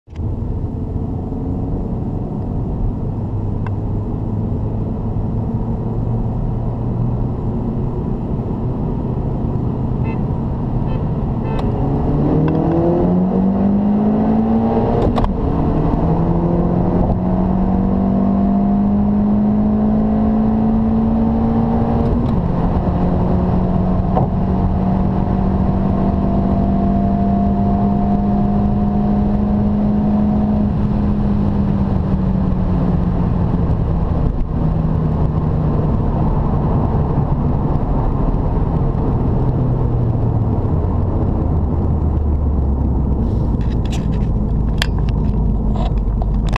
Scirocco 1.4 tsi revo vs Scirocco 1.4 tsi etuners part 2
Scirocco camera: Revo stage 2, custom filter, custom exhaust, i/c s3
Scirocco black: Etuners, milltek turbo-back, Apr carbonio, i/c s3
Roll 2nd gear 60km/h
Rec: gopro hero 3 white edition 720p